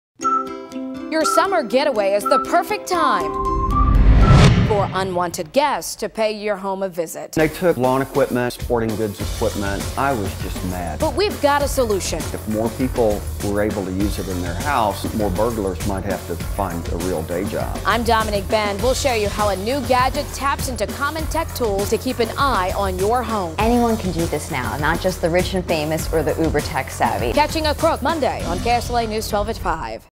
CATCHING A CROOK
We'll show you how you can keep an eye on your home from your smartphone! Don't miss our special report, Monday at 5pm.